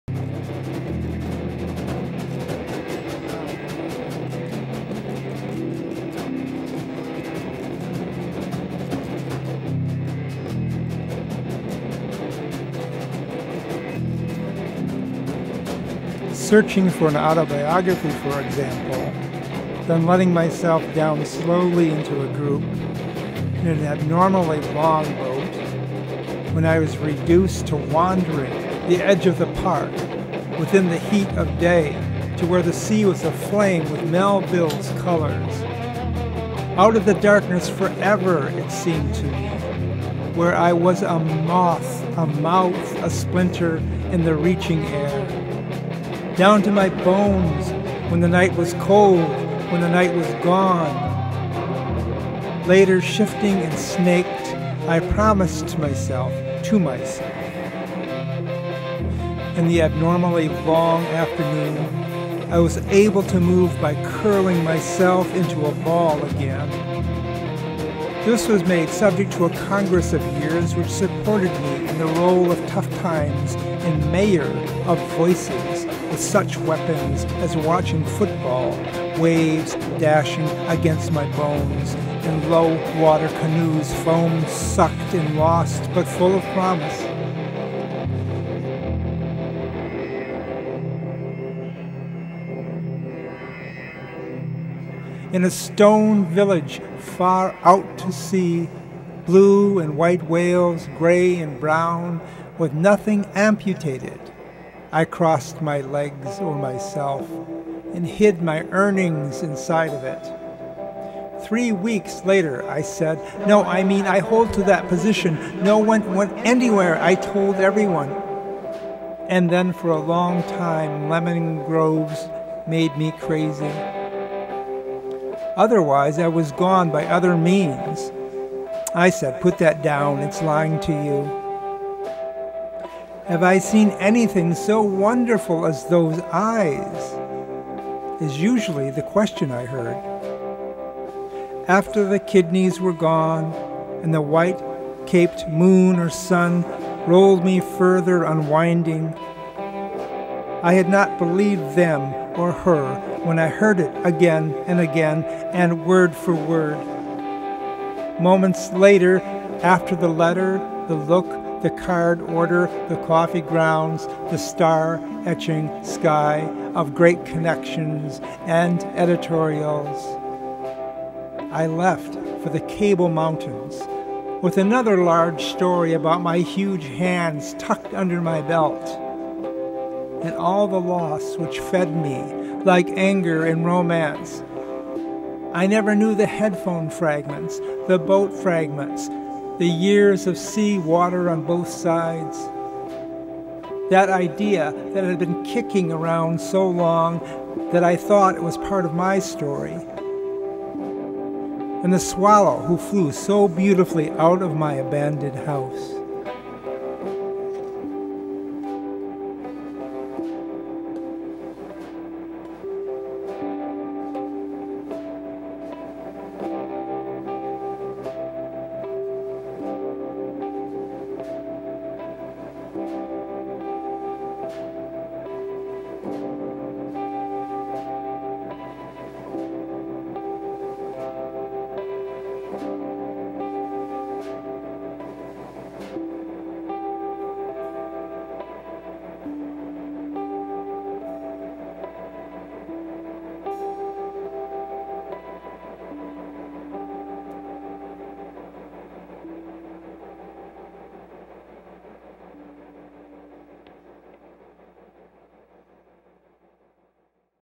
Searching For An Autobiography
Poem by George Farrah from Low Pouring Stars, published by Ravenna Press.  Bird People by Tom Cassidy.  Landscapes by George Farrah.  Music from Farmer Farmer.  Animation by Wayne Nelsen.